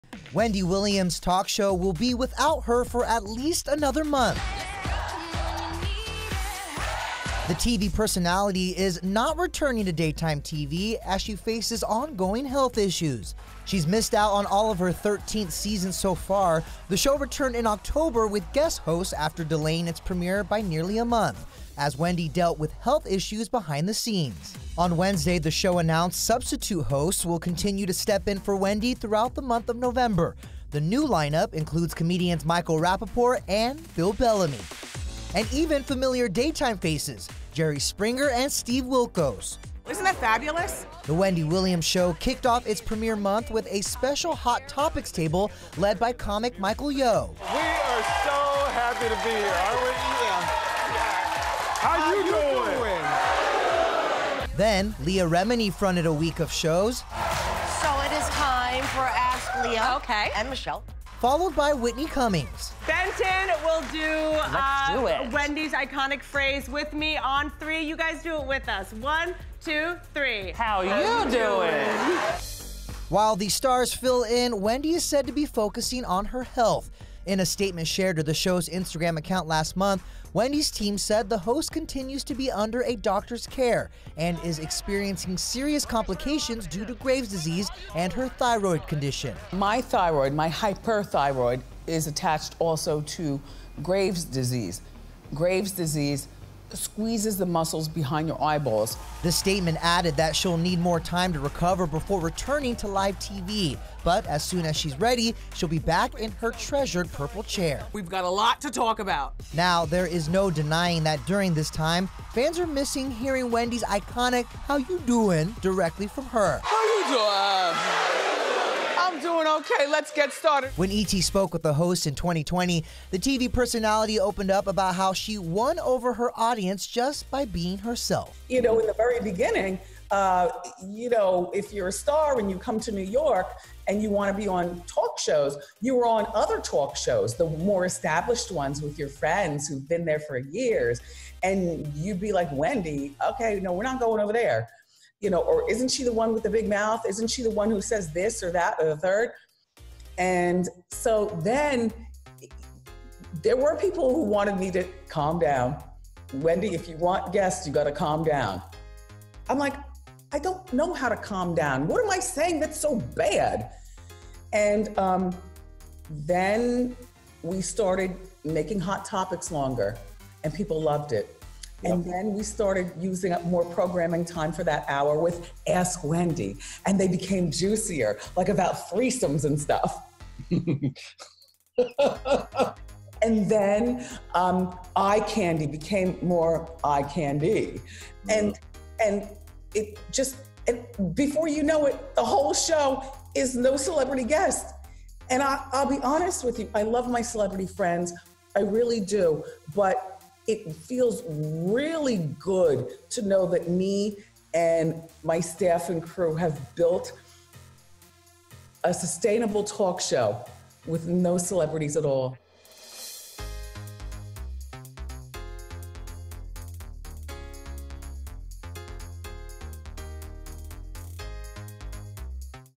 Wendy Williams NOT Returning to Talk Show for at Least Another Month
Wendy Williams is taking more time off from her talk show, ‘The Wendy Williams Show,’ as she faces ‘ongoing medical issues.’ The production team behind the show announced that guest hosts will continue to fill in for Wendy throughout the month of November. In a statement shared to the show’s Instagram account last month, it was revealed that the 57-year-old TV personality continues to be under a doctor’s care and needs more time to make a full recovery.